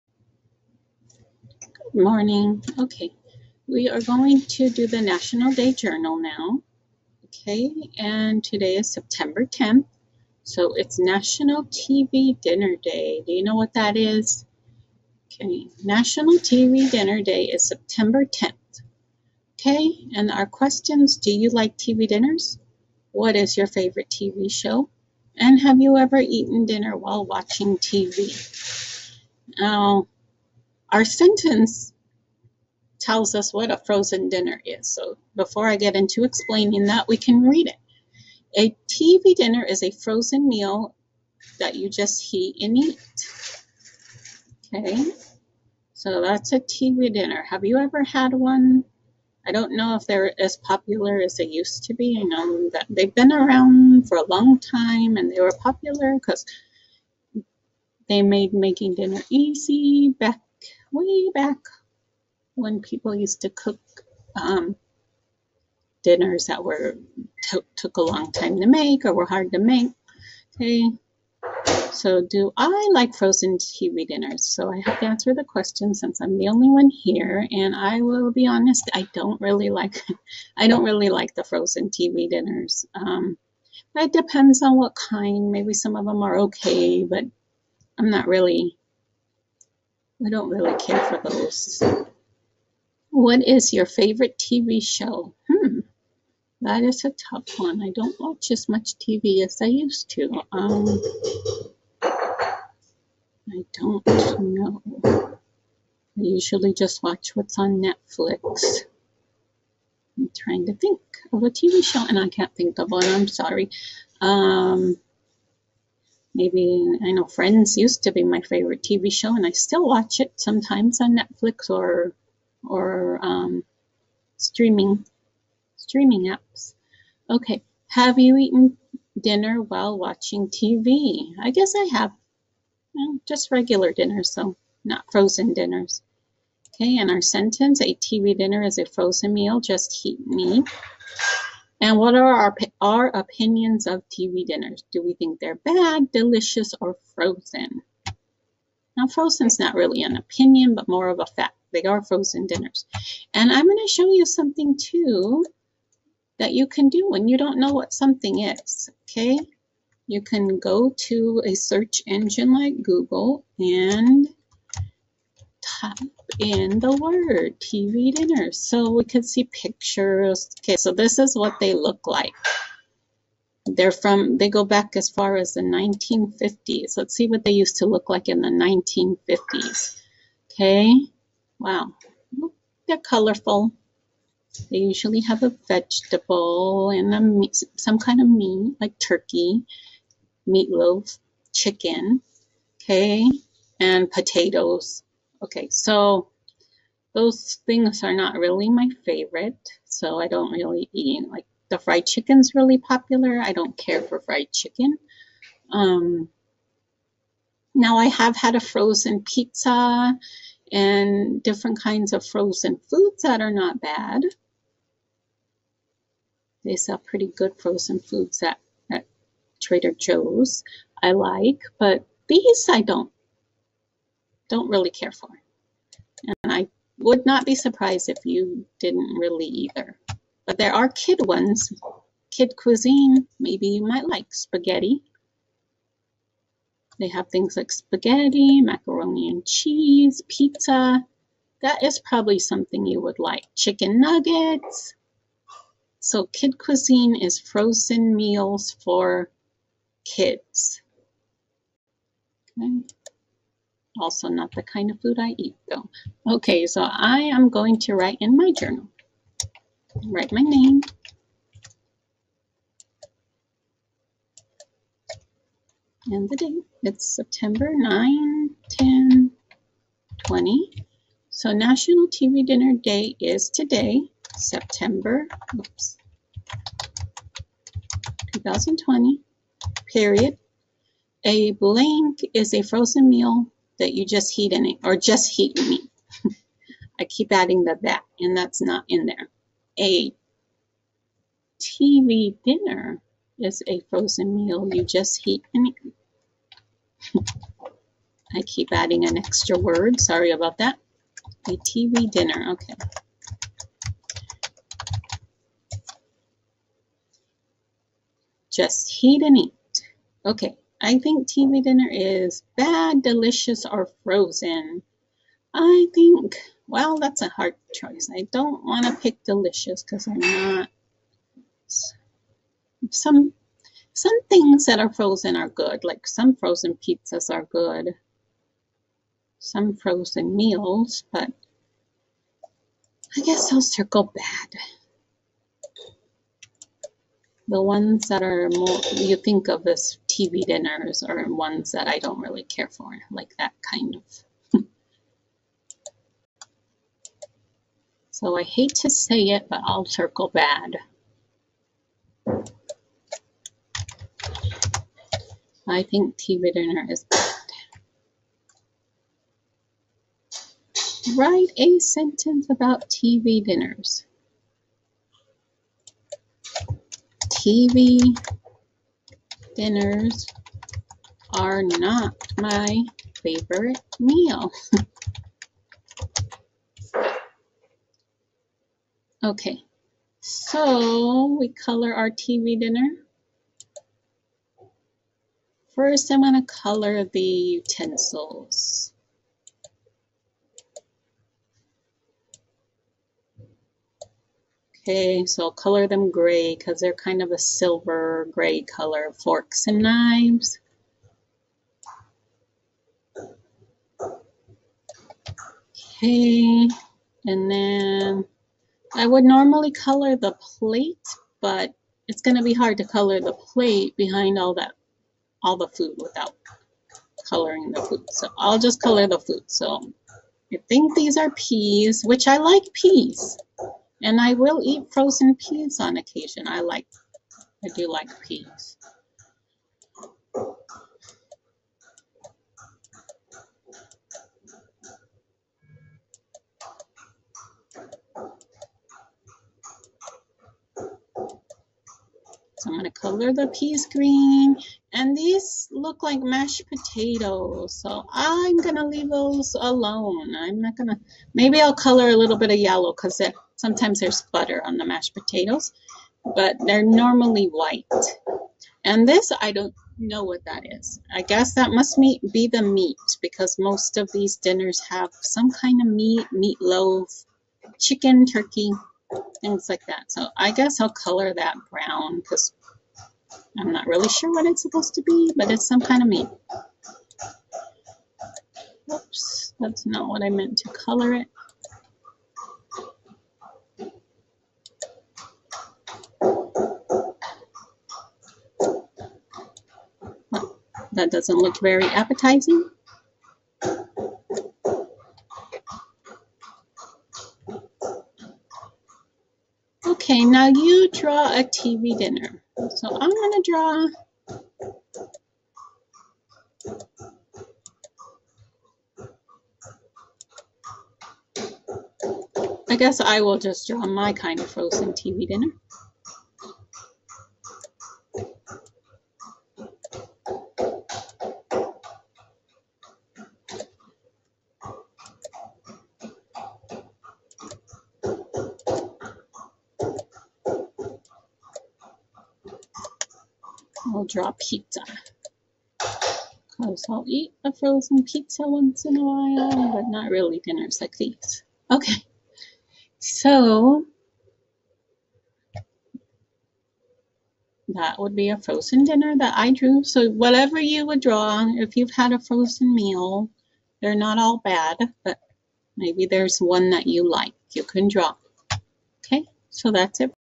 Writing Lesson-National Day Journal for Thursday, September 10, 2020
Here is a writing lesson. I am completing the National Day (Level 3) Journal page for September 10, 2020.